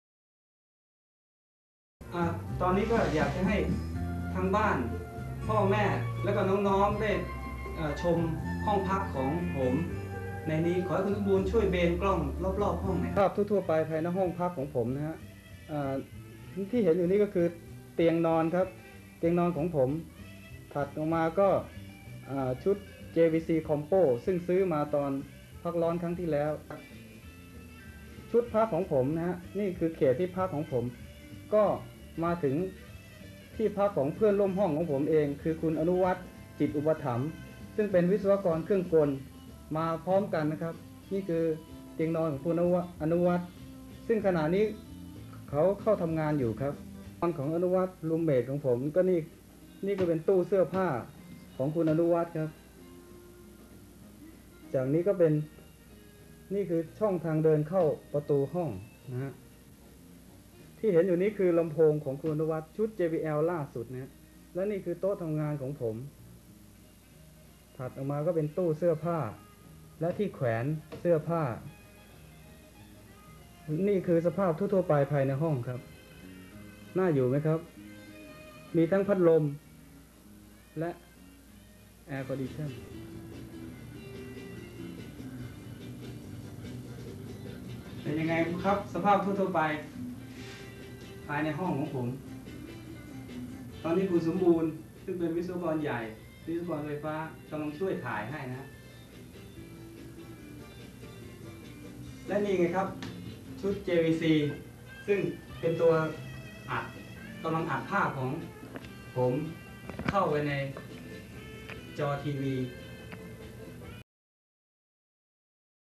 Prasong's life in Jeddah Episode 3
My memory to work in Saudia Catering,
Jeddah, Saudi Arabia from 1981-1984
Episode 3    “In the room”